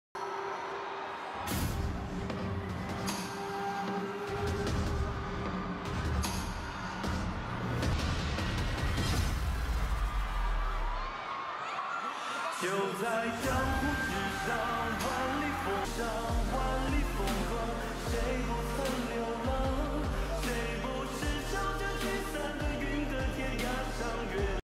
Kind gesture of Dylan Wang, picked up a bottle of water for Victoria without being asked
These two siblings are so cute!